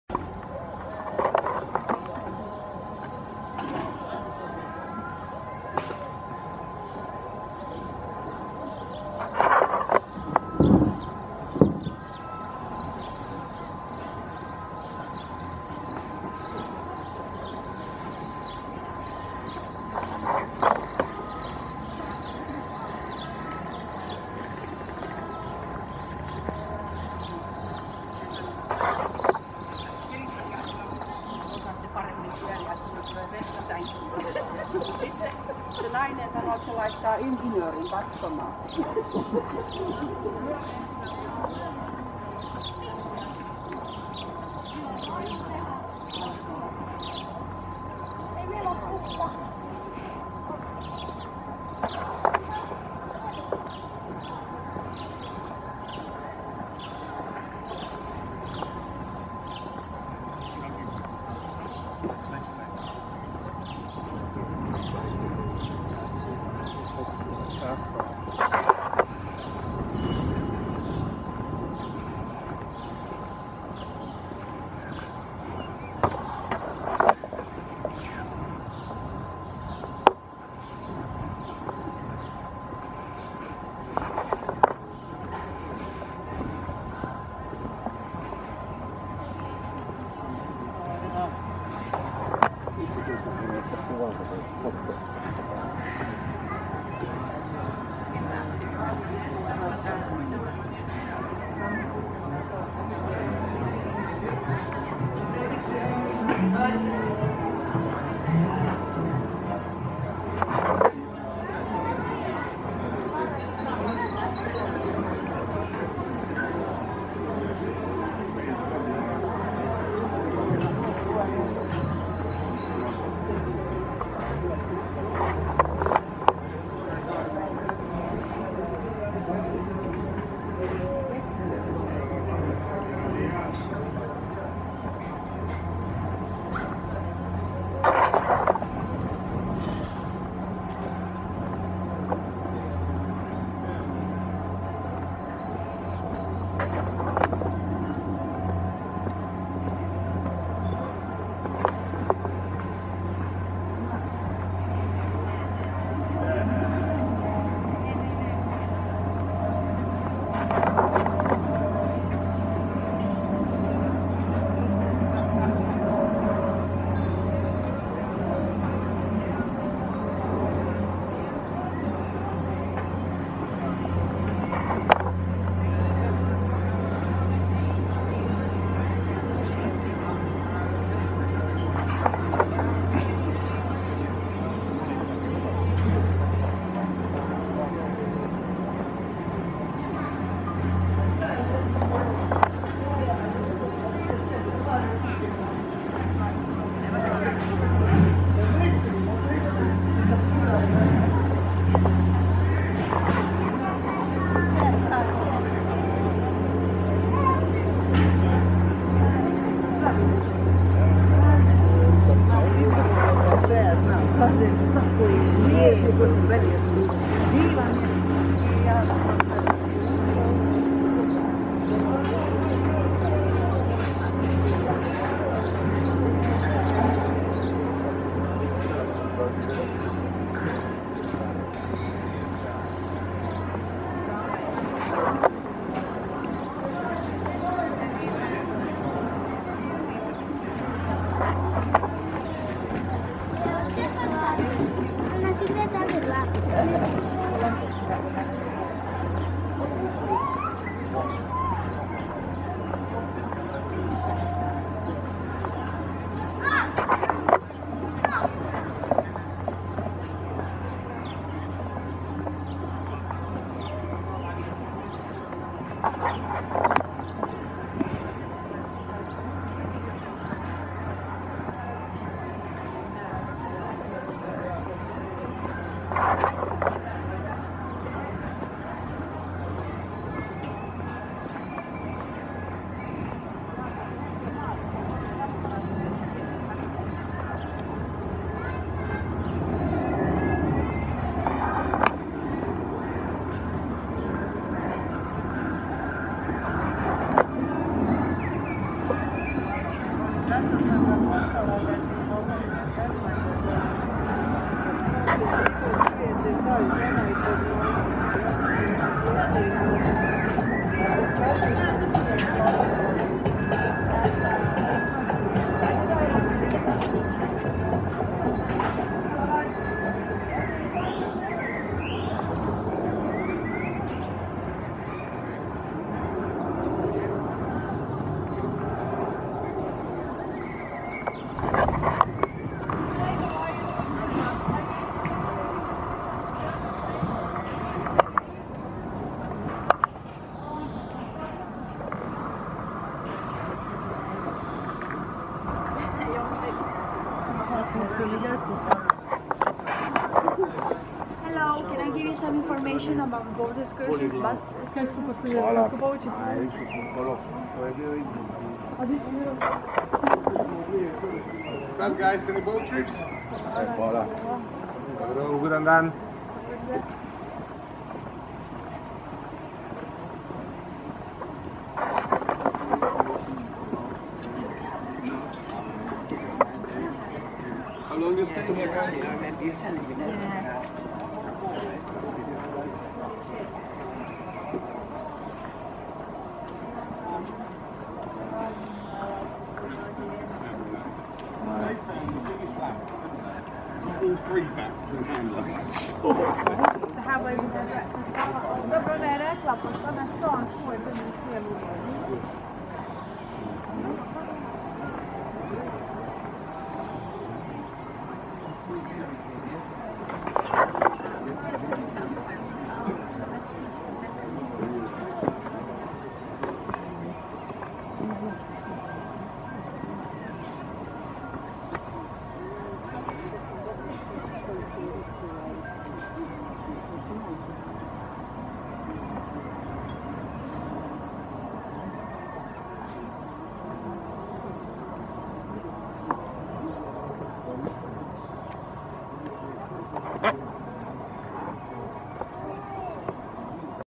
Lapad in Dubrovenik Croatia 01
The Virtual Tourist walks  around Dubrovenik harbour in Croatia